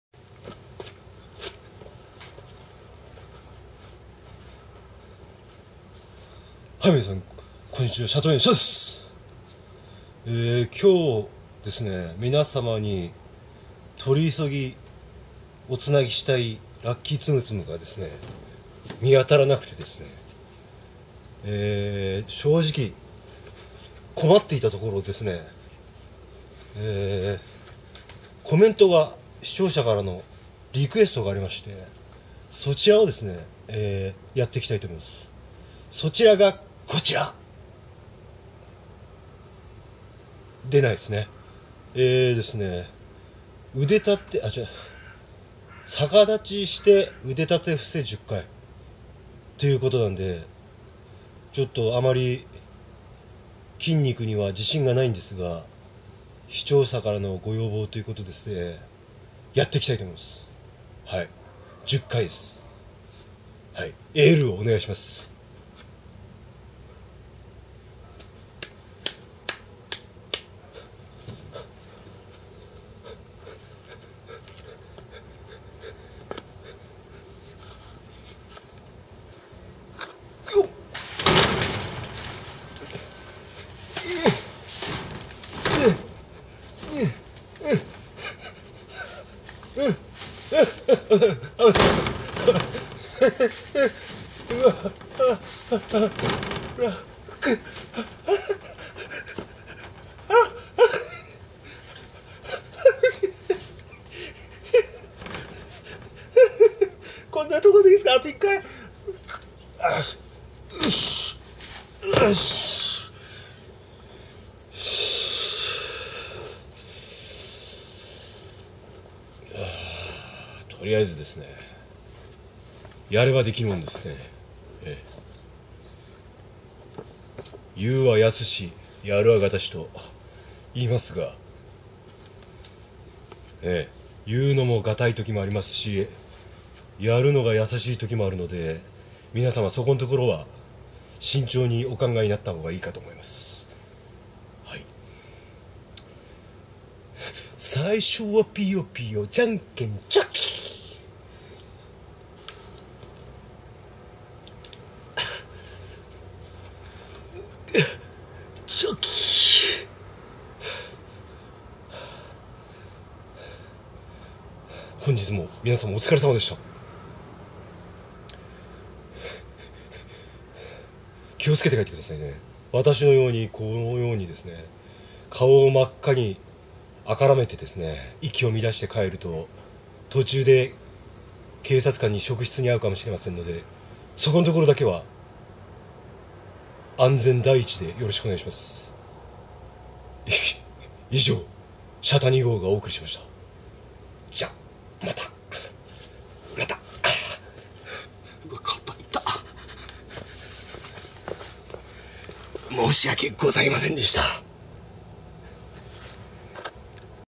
リクエストにお答えします！本日はお疲れさまでした！／Respond to the request ! Today thank you !／きゅうり農家／きゅうり栽培／愉快なshata農園
どうも！シャタ２号です！
何のこっちゃ分かりませんが、
逆立ち腕立て伏せ頑張りました！

Respond to the request ! Today thank you !